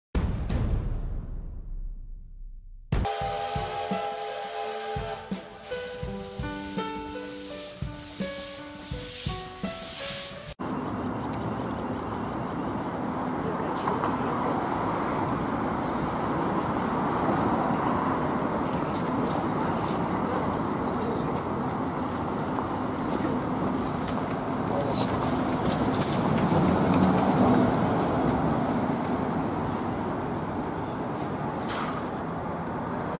[90х] Троллейбус с УАХ БКМ 321100D №2113
Троллейбус с УАХ БКМ 321100D №2113 (Приписан ГУП "ГорЭлектроТранс",Троллейбусный парк №2)
Маршрут №43 -
"Товарищеский проспект - Финляндский вокзал,ст.м."Площадь Ленина"".

Площадь Ленина,Санкт-Петербург,Россия.
24.Х.2020